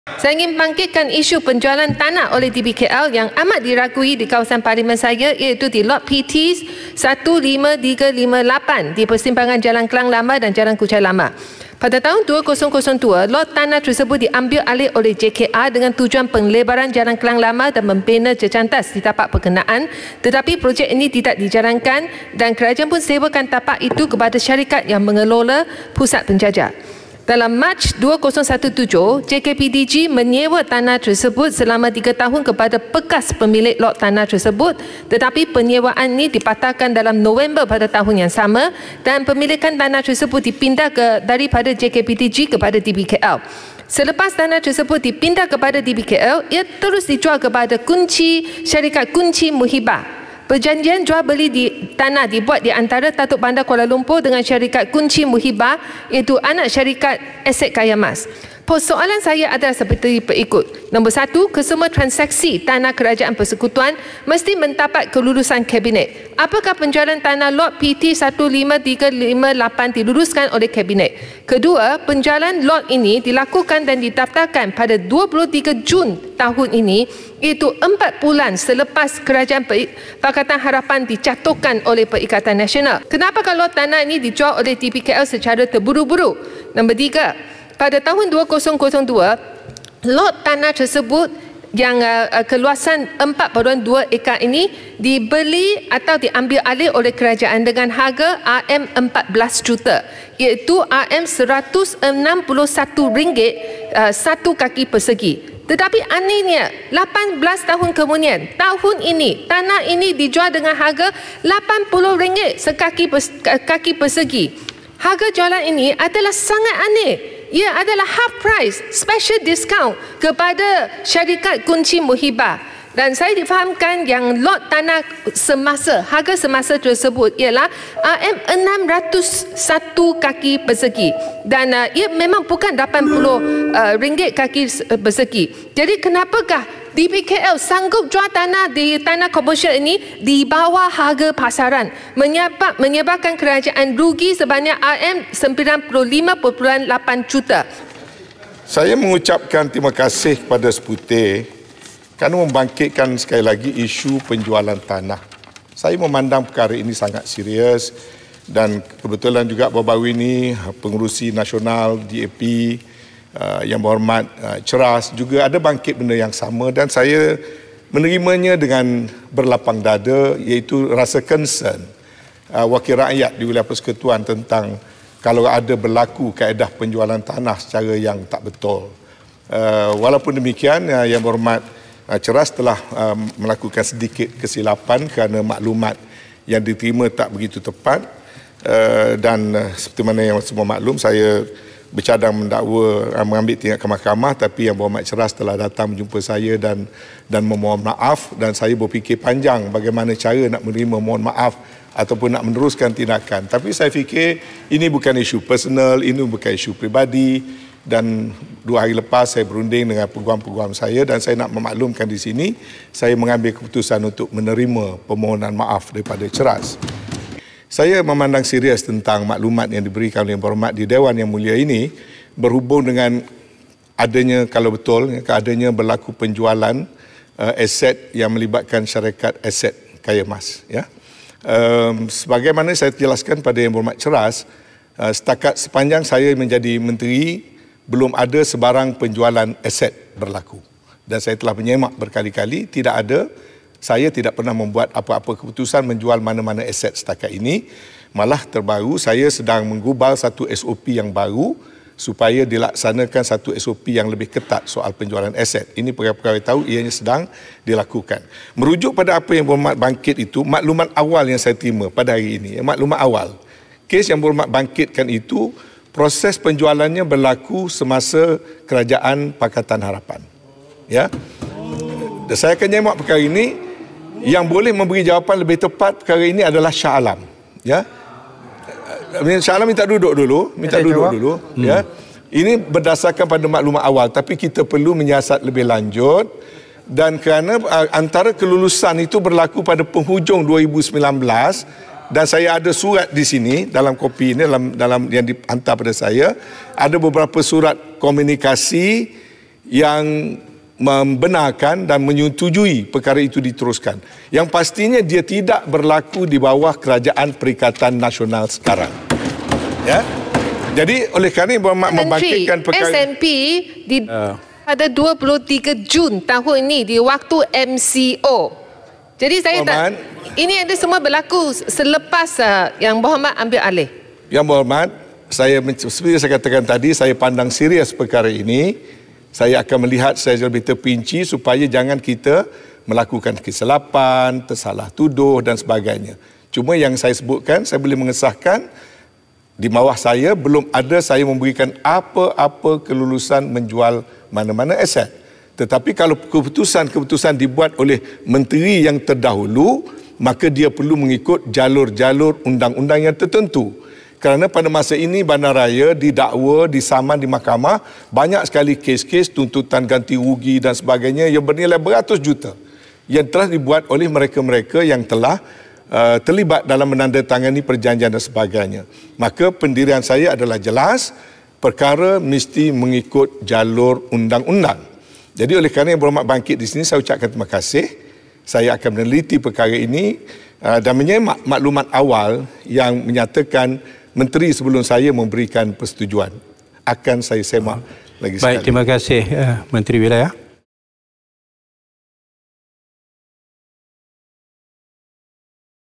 Land sale: FT Minister accepts Cheras MP’s apology, Seputeh MP raises another case
Federal Territories Minister Tan Sri Annuar Musa said he has accepted Cheras MP Tan Kok Wai's apology for the latter's claim that some 42 assets belonging to Kuala Lumpur City Hall were sold during Annuar's tenure.

Annuar said this after Seputeh MP Teresa Kok raised another concern over the sale of a piece of land at the junction of Old Klang Road and Jalan Kuchai Lama.